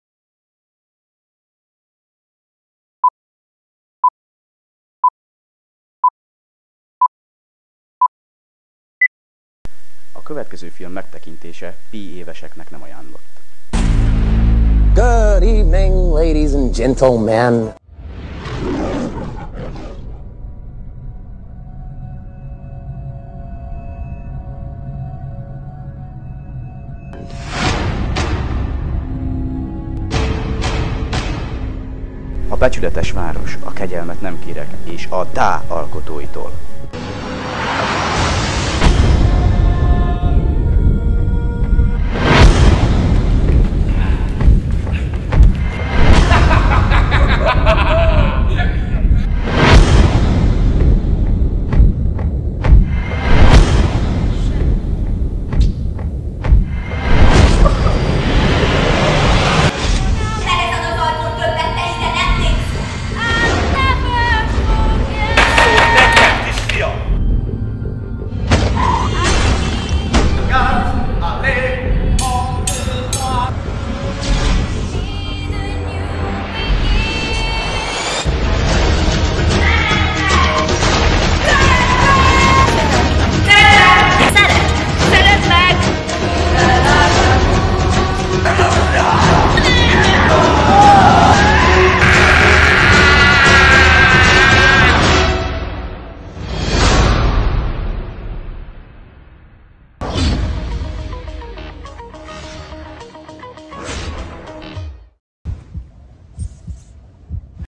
MULTILINGUAL ÜBEROPERETT TRAILER
Csinálunk egy Multilinguál Überoperett című filmet Keserű tanár úrral mi, a Horváth Mihály Gimnázium 12.dá. osztálya.
Én meg, csináltam neki előzetest. 1 perc 47 mp lett. Tessék.